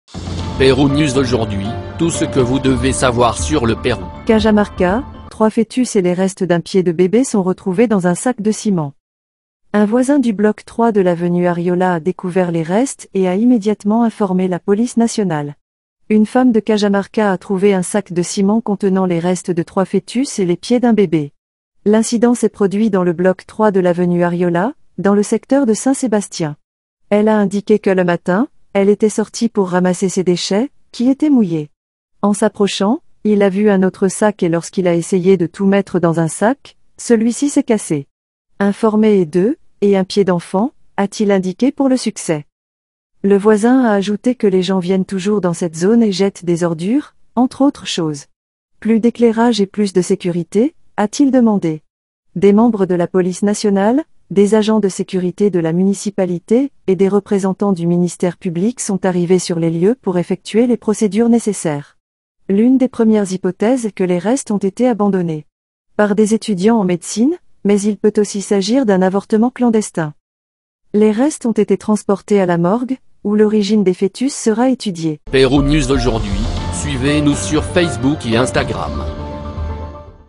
🇫🇷 Cajamarca : Trois fœtus et les restes d'un pied de bébé sont retrouvés dans un sac de ciment.
Un voisin du bloc 3 de l'avenue Arriola a découvert les restes et a immédiatement informé la police nationale. Une femme de